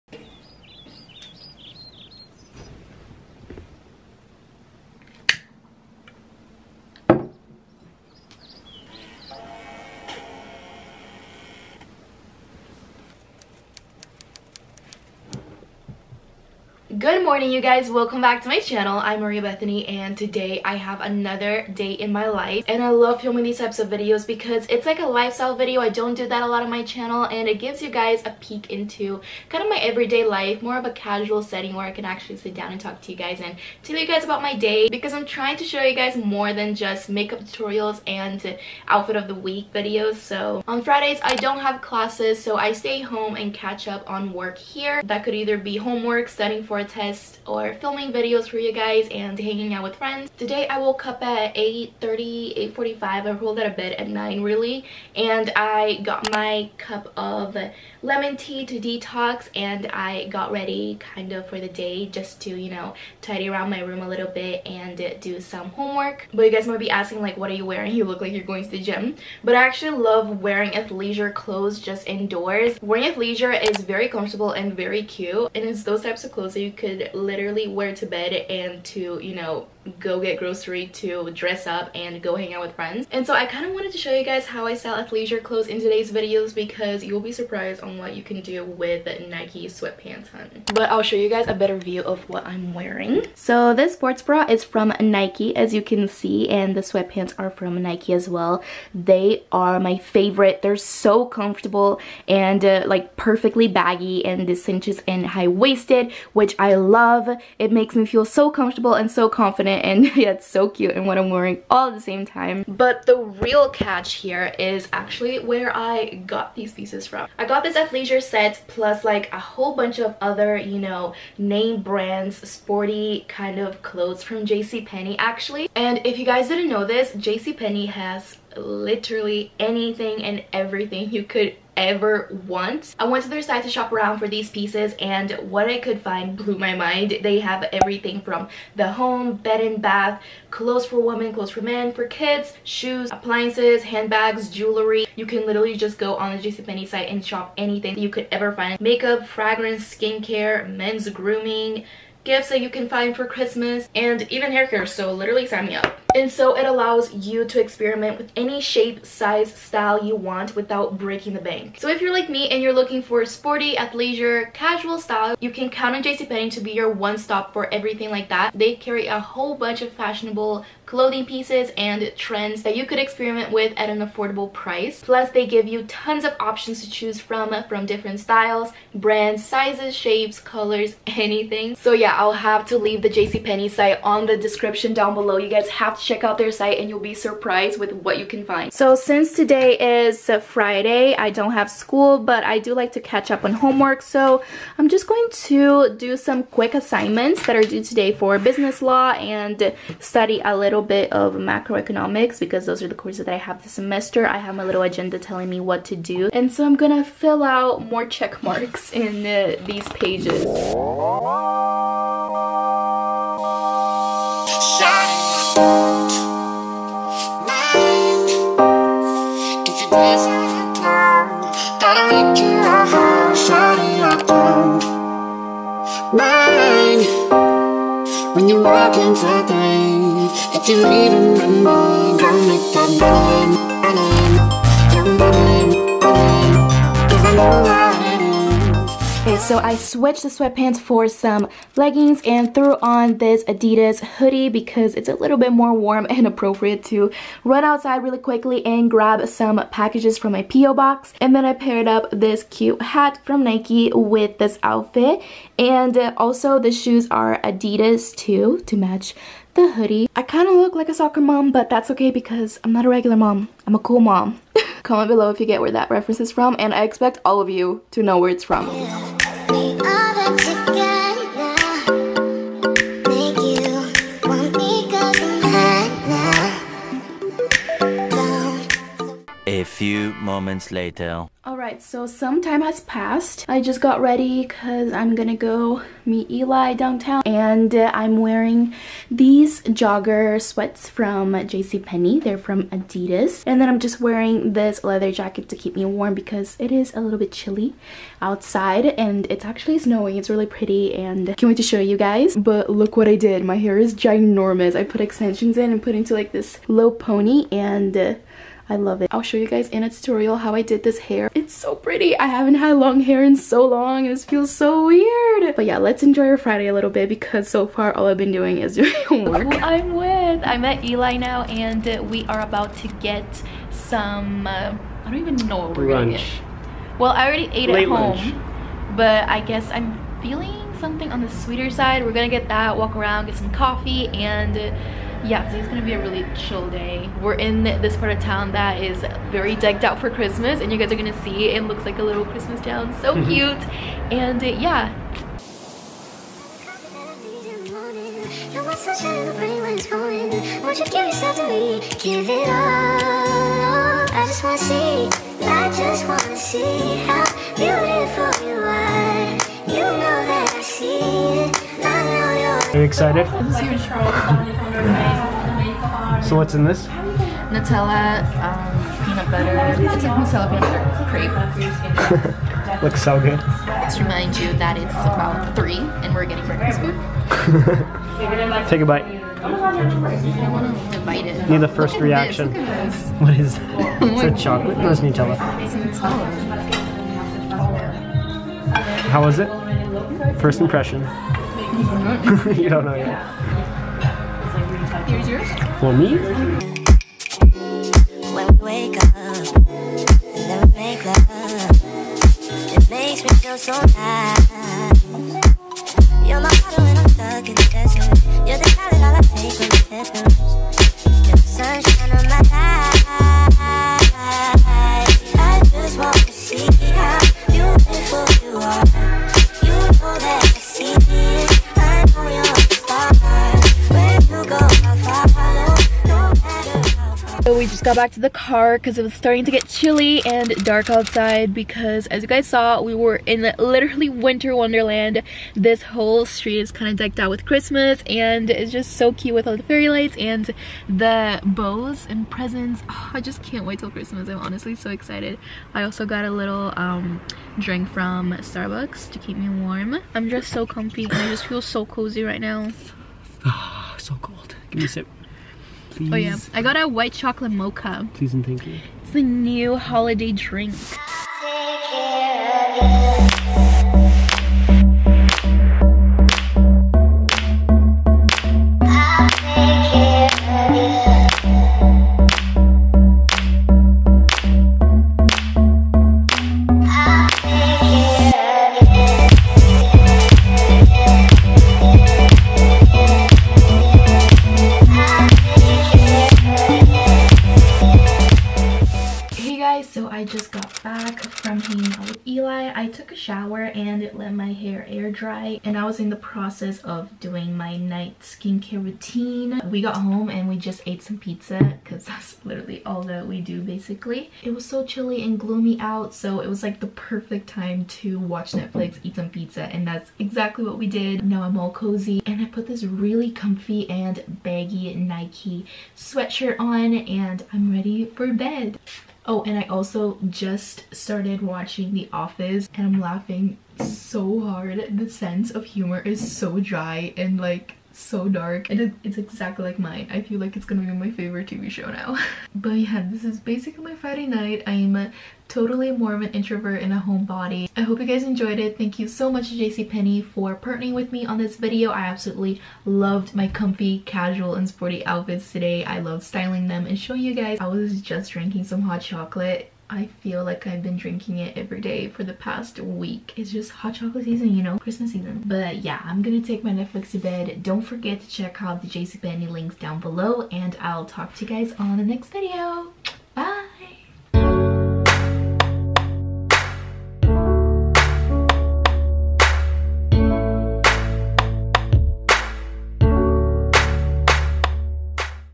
A DAY IN MY LIFE: WEEKEND EDITION! | Maria Bethany
HELLO EVERYONE ♡
I'M BACK WITH ANOTHER DAY IN MY LIFE! I'M SO EXCITED YOU GUYS ARE ENJOYING THIS VIDEO, THIS ONE IS A BIT DIFFERENT SINCE YOU GUYS WILL FOLLOW ME AROUND A CHILL BUT FUN FRIDAY!

ENJOY xx